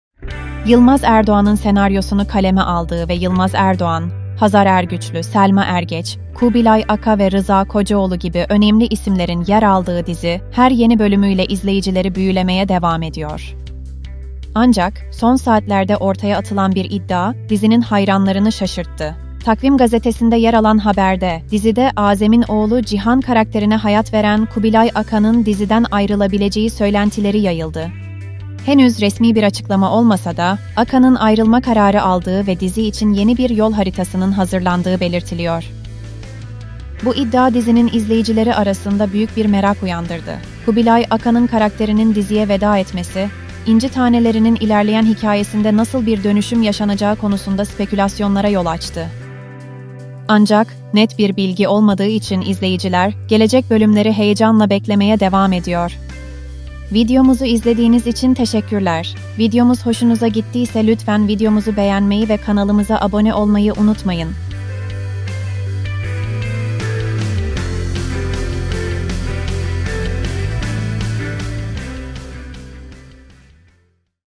İnci Taneleri'nde Şok Eden Ayrılık! Başrol Oyuncusu Veda Ediyor!
Yılmaz Erdoğan'ın senaryosunu kaleme aldığı ve Yılmaz Erdoğan, Hazar Ergüçlü, Selma Ergeç, Kubilay Aka ve Rıza Kocaoğlu gibi önemli isimlerin yer aldığı dizi, her yeni bölümüyle izleyicileri büyülemeye devam ediyor. Ancak, son saatlerde ortaya atılan bir iddia, dizinin hayranlarını şaşırttı.